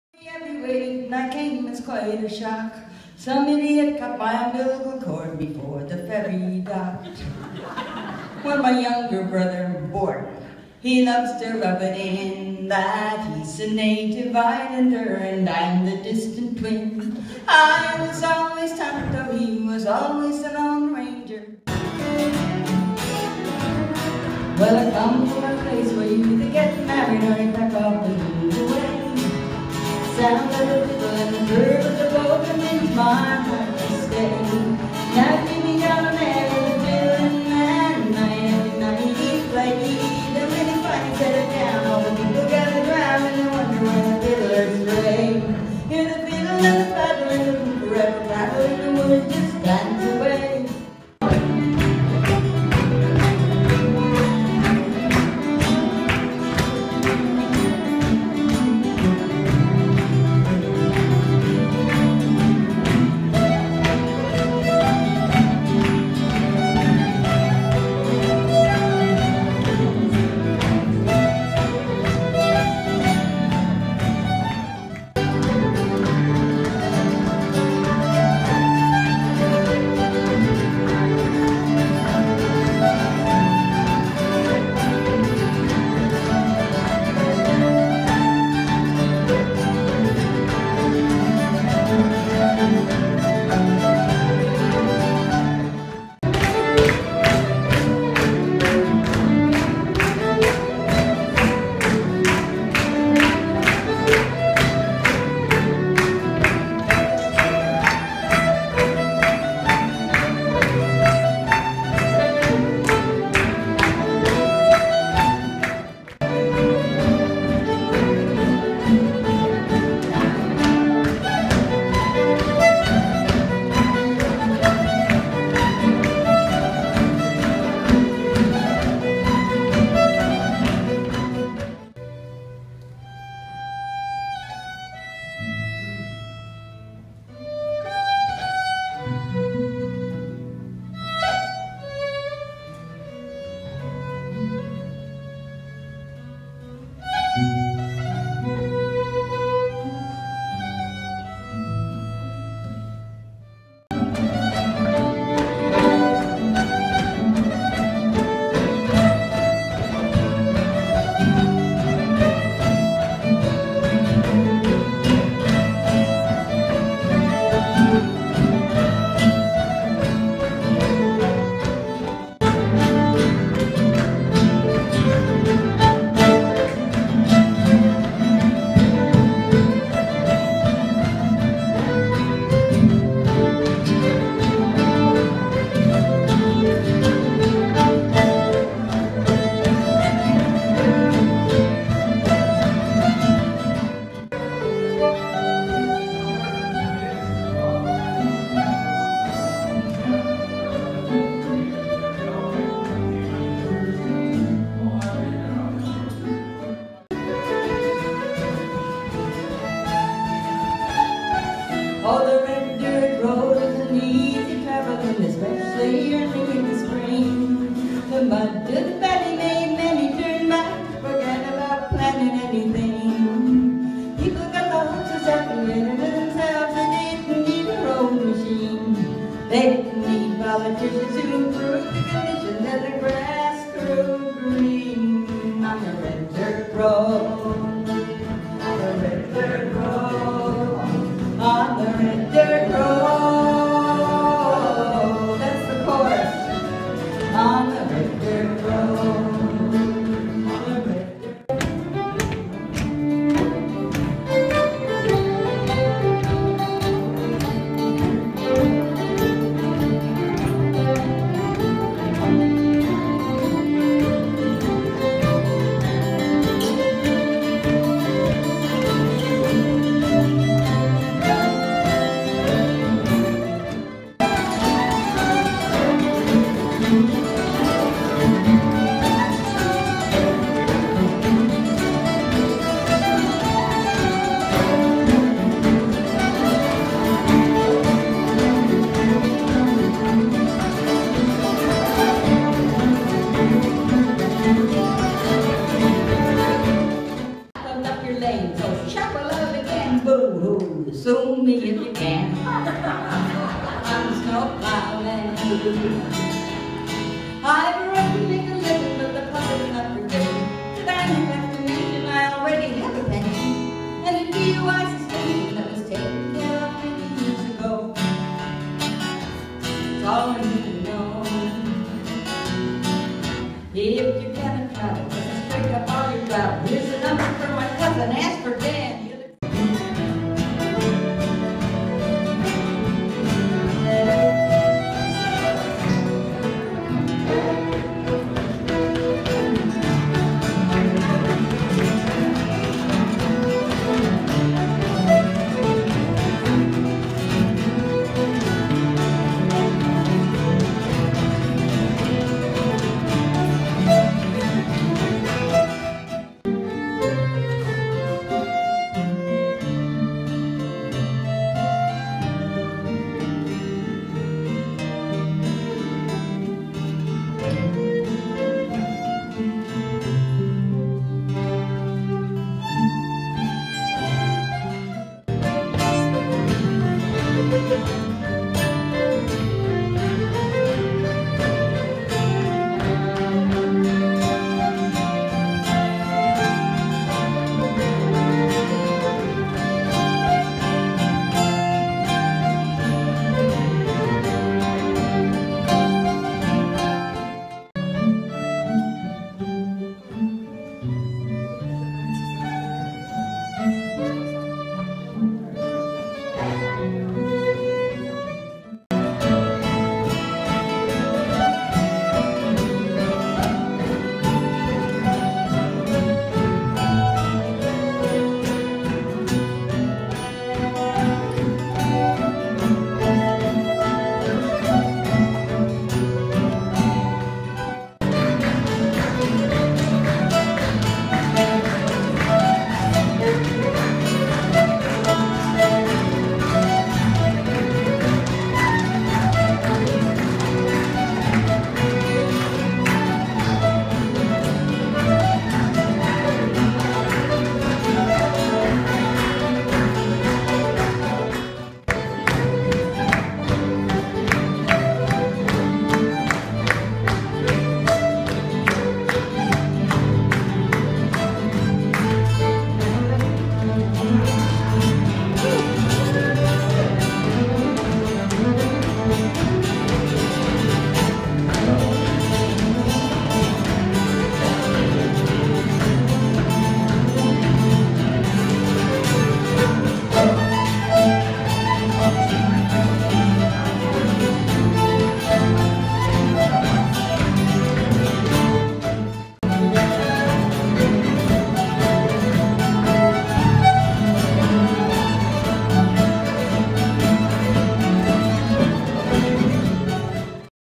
Irish Hall Ceilidh
A ceilidh at the Irish Hall in Charlottetown, PEI.